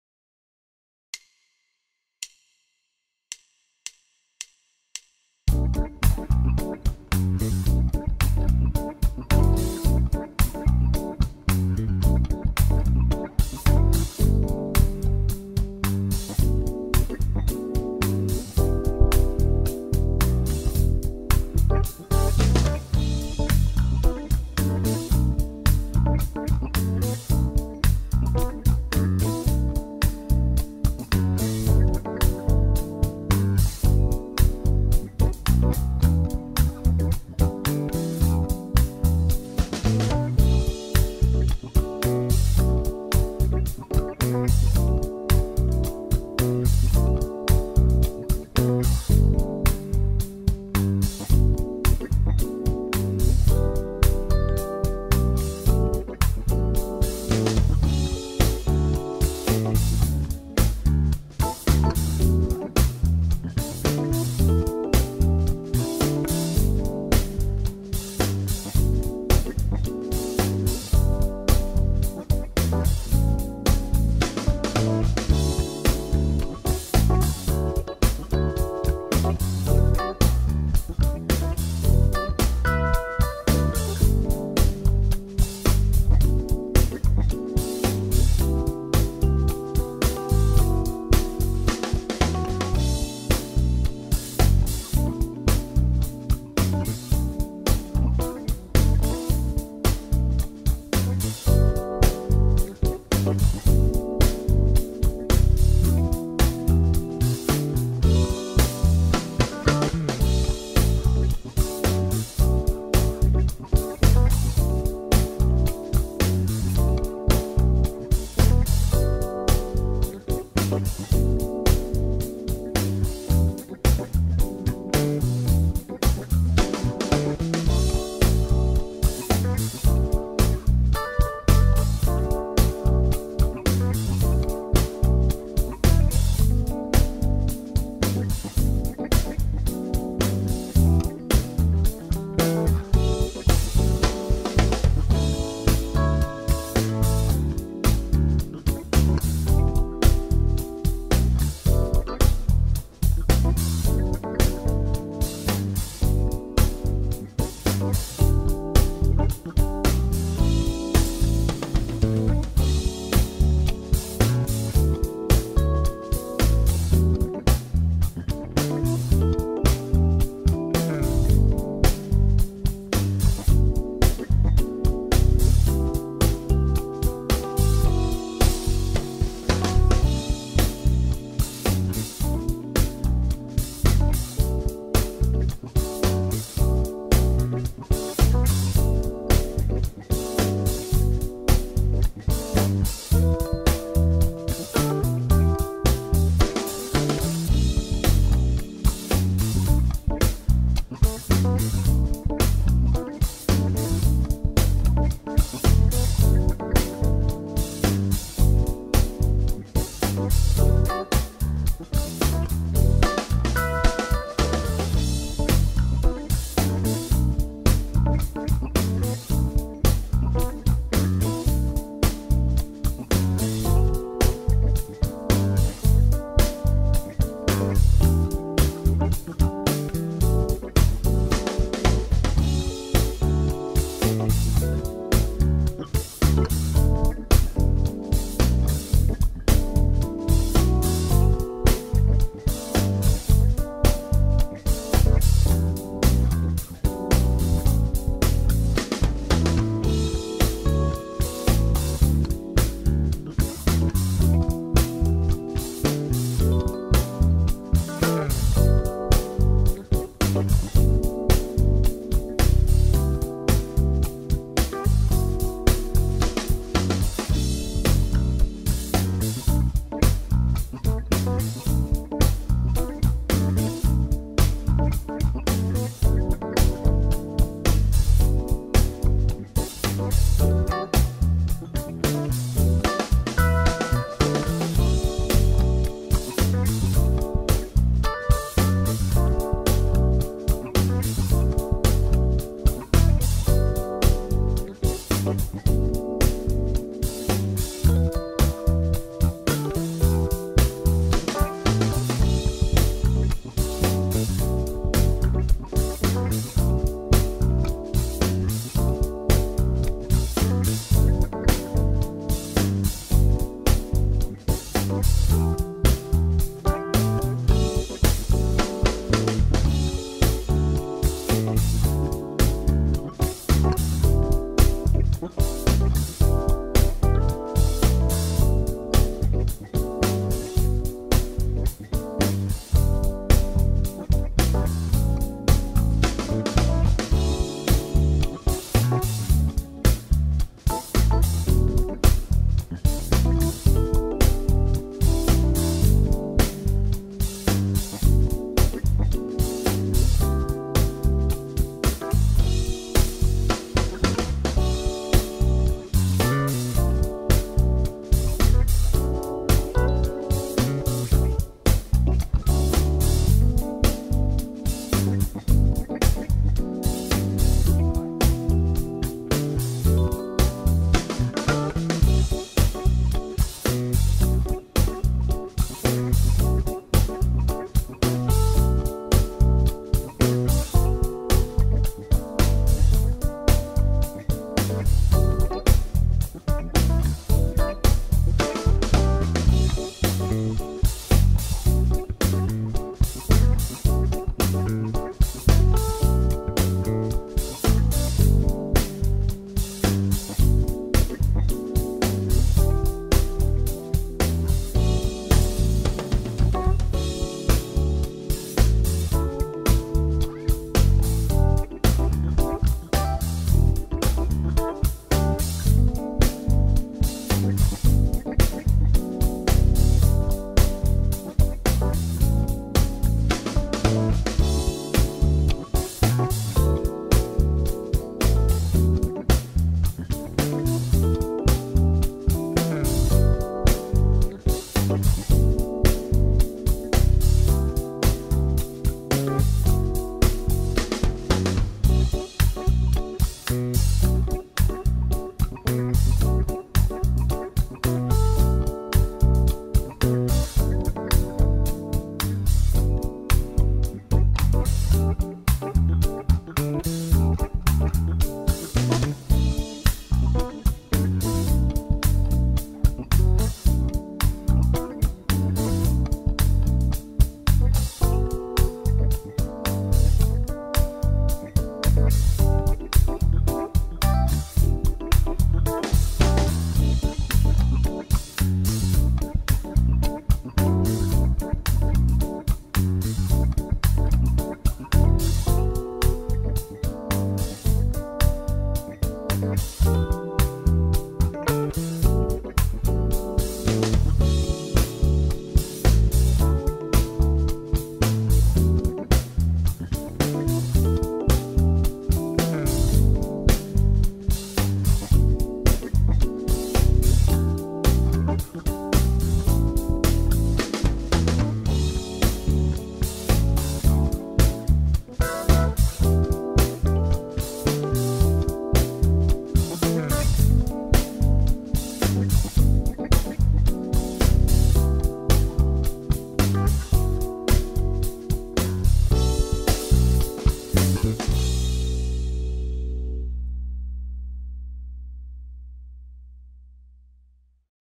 Lively Dance Jazz Funk TRIO Backing Track in Bm
CHORD PROGRESSION
IS FIXED IN THE
COMMENTS.

Did not find the backing track you are looking for? Leave us a suggestion in the comments and we will do our best to make it available on our channel.